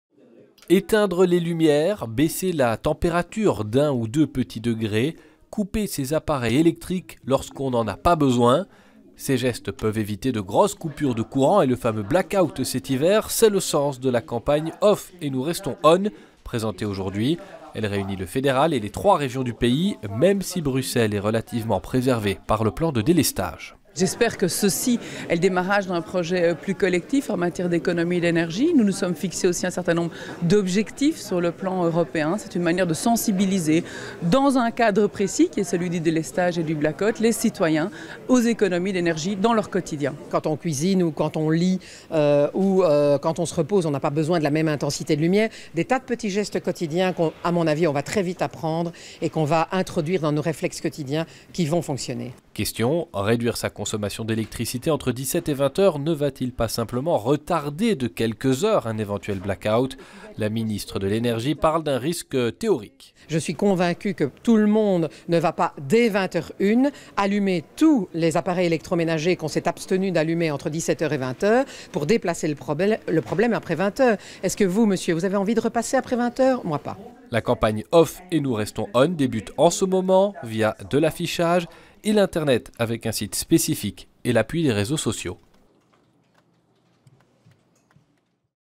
Campagne "Off pour rester On" pour éviter le "black out" énergétique
Le fédéral et les régions ont lancé la campagne "Off pour rester On". Les petits gestes qui permettent d'économiser l'électricité et éviter un "black out" électrique.